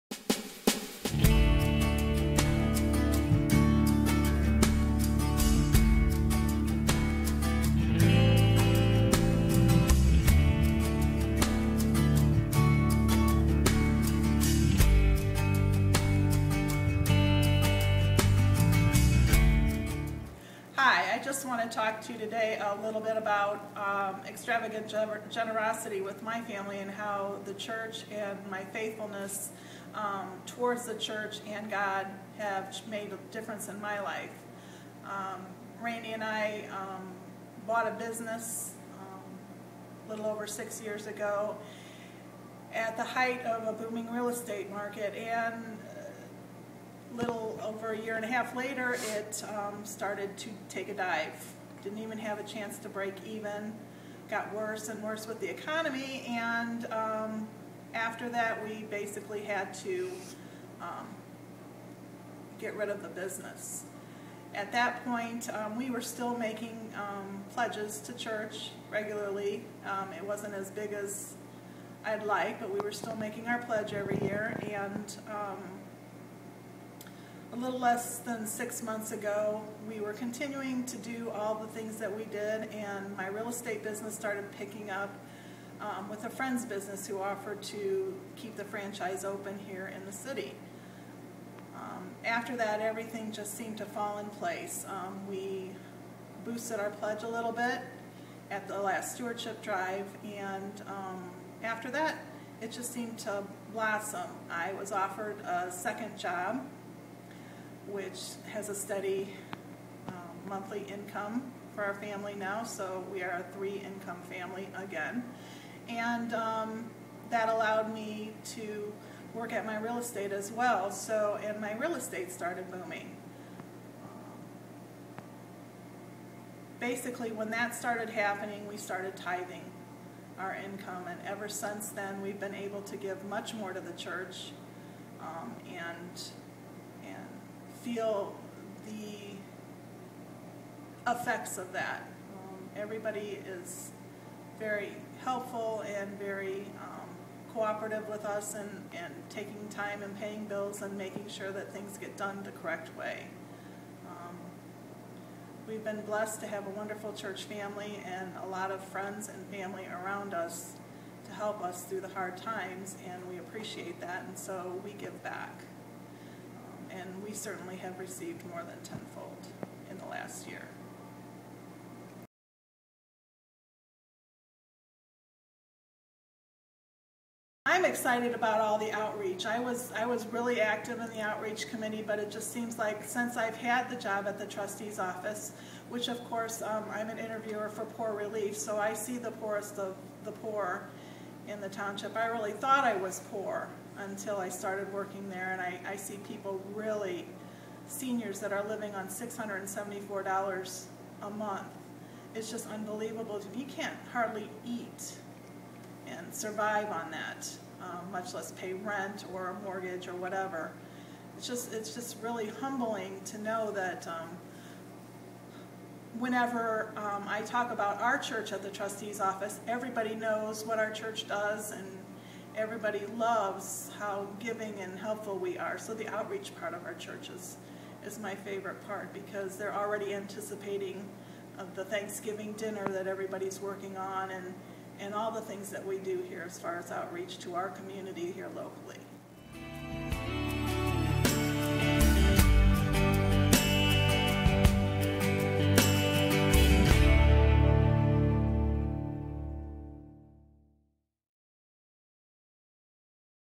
First Stories: Robin Wilkening
First of two videos for our 2011 Generosity campaign. Robin Wilkening talks about her family's experience with extravagant generosity.